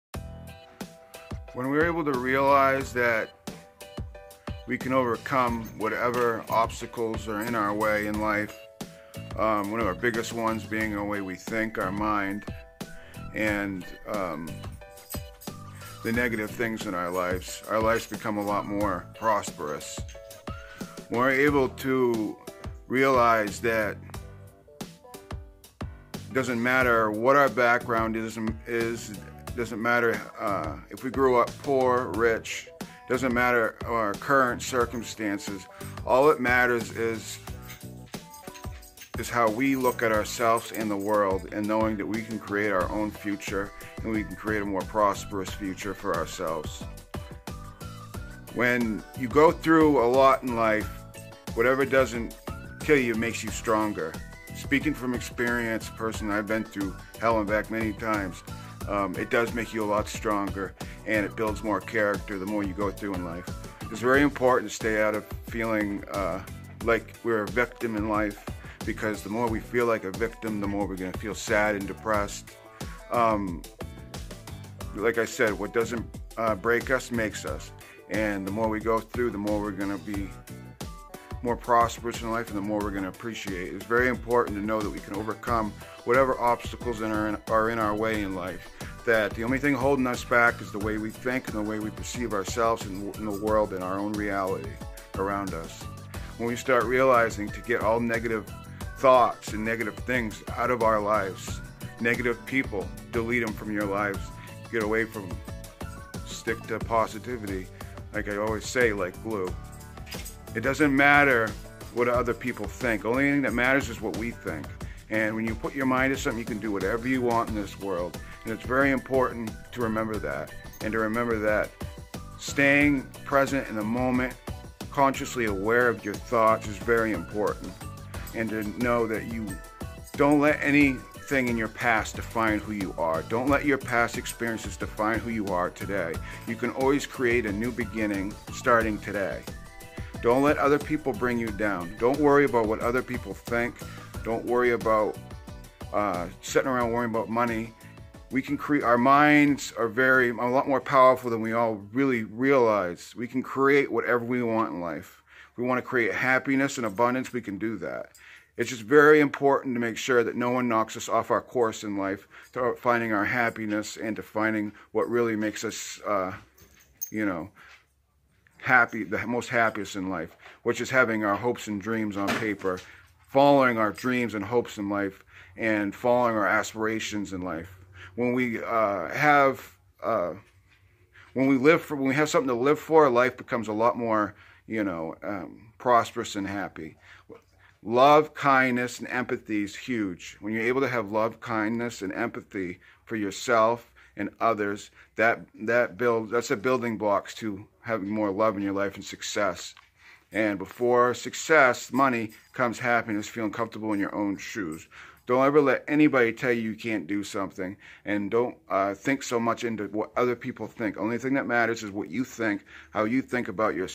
Eliminating negativity from our everday Lifes.
Eliminating negativity From our lifes.
Getting Past all the negativity and obstacles in life. Staying positive .